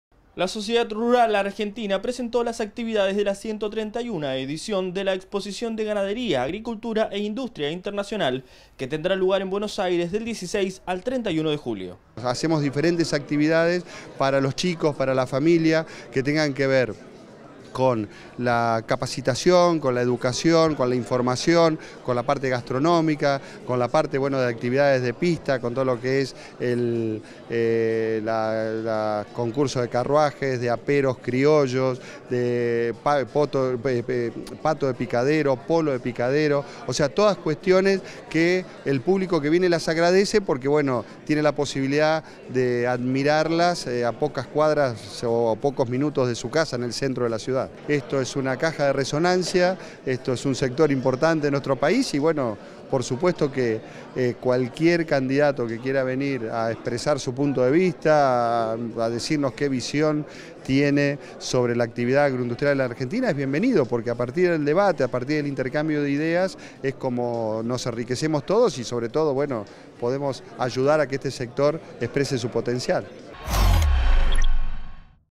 El campo celebra la edición 131 de la Exposición Rural
Del 16 al 31 de julio se lleva adelante éste evento que acerca el campo a la ciudad. Con actividades para toda la familia,
la exhibición trae además cultura y tradición representadas en 13 provincias y más de 60 expositores.